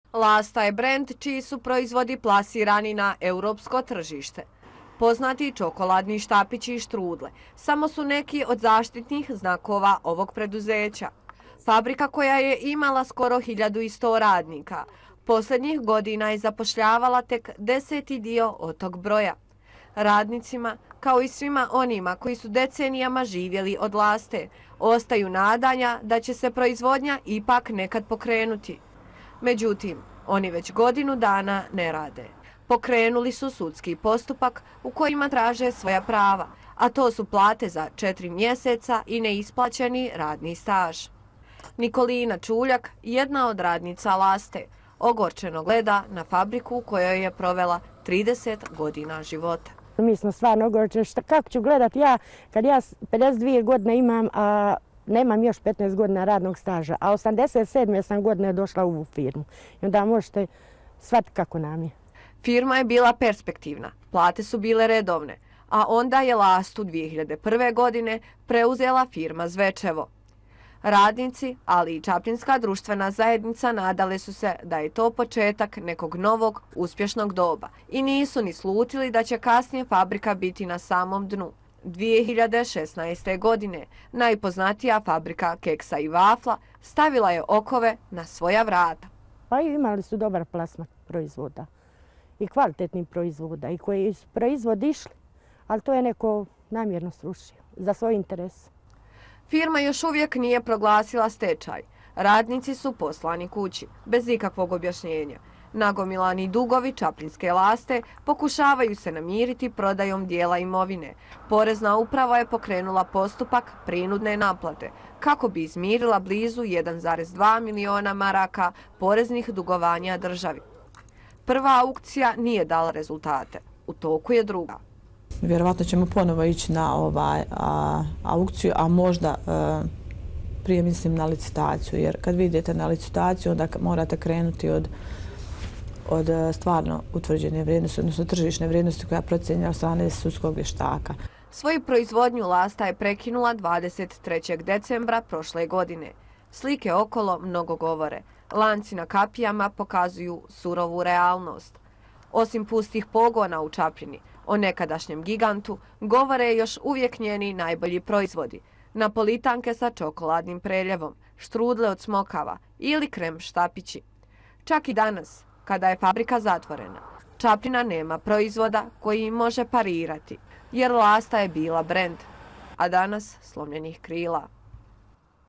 Donosimo priču o „Lasti“ iz Čapljine – City televizija
Čapljinska Lasta, bila je nadaleko poznata fabrika keksa i vafla. Fabrika koja je jedan od simbola male općine na samom jugu naše zemlje, prije rata zapošljavala je više od hiljadu radnika, radila i u ratu, a u posljednjem desetljeću skliznula je na samo dno.